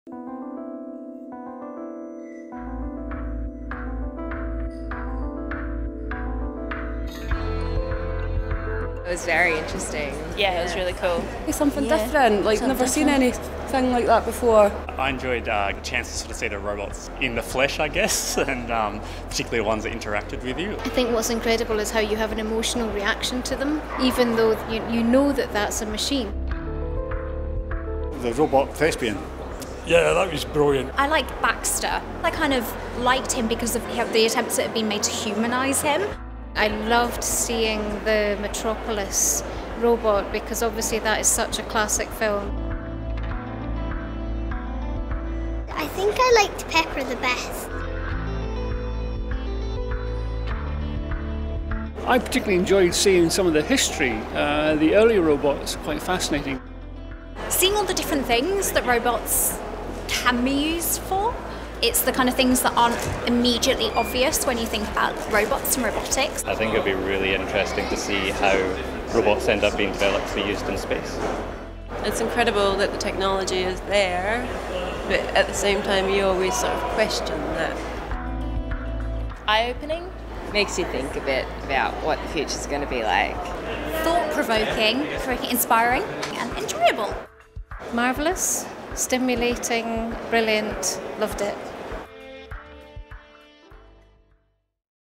Robots at the National Museum of Scotland: Visitors' views
'Eye-opening', 'Inspiring', 'Brilliant'

These are just some of the ways visitors have described the Robots exhibition, now on at the National Museum of Scotland until 5 May 2019.

Don’t miss your chance to see the most significant collection of humanoid robots ever assembled, from early automata to science fiction film stars and the creations of modern research labs.

See early clockwork machines, a modern recreation of renowned British robot Eric, and stars of the silver screen, including a T-800 endoskeleton used in the movie Terminator Salvation and a replica of Maria from the iconic 1927 film Metropolis.

Developed by the Science Museum, London.
Supported by players of People's Postcode Lottery.